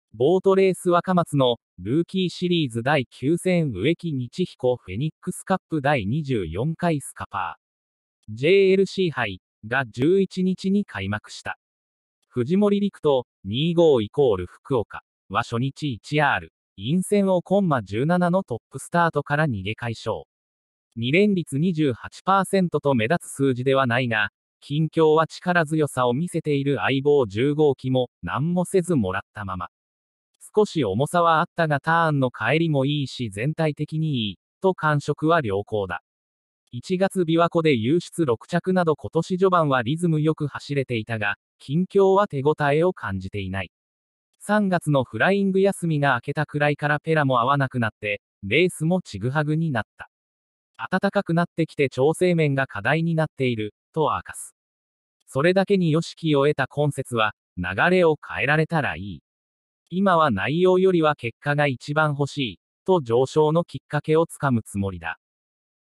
【若松ボート・ルーキーＳ】藤森陸斗が白星発進「流れを変えられたらいい。内容より結果が欲しい」[Japanese sports]
Iowa